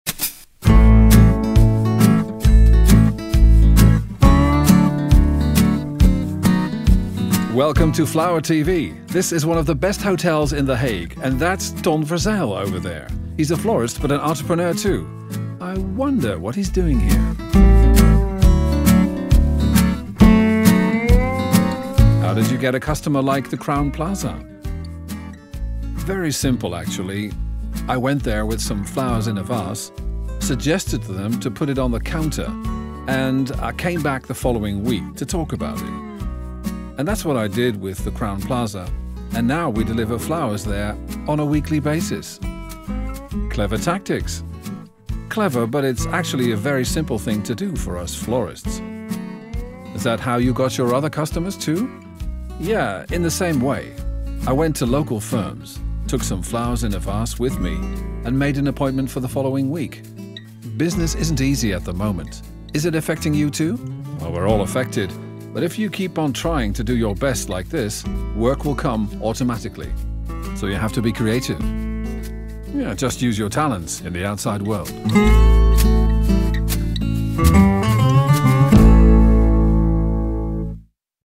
FlowerTV: aflevering 1
Laat de bloemen voor je werken: Ton Verzijl legt uit hoe.